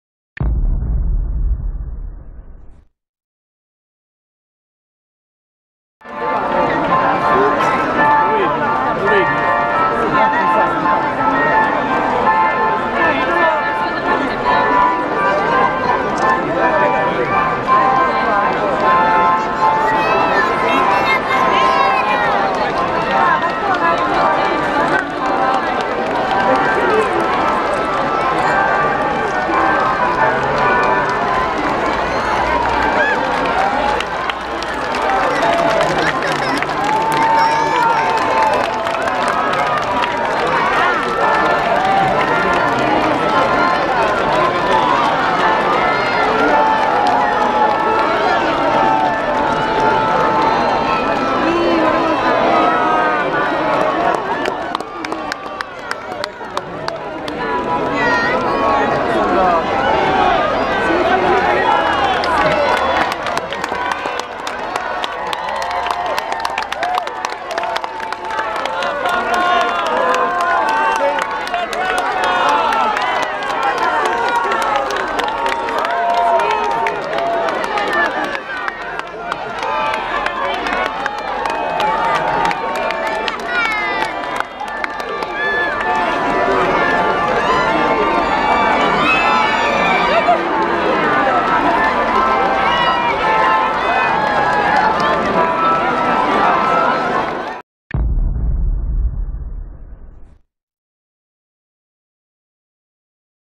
Pope FRANCIS pass me NEAR _ GoPro Hero 3+(Super view)
Pope Francis shooting in conciliazione street after mass on pope car..

SHOOT IN HD (new) GoPro HERO 3+ Super View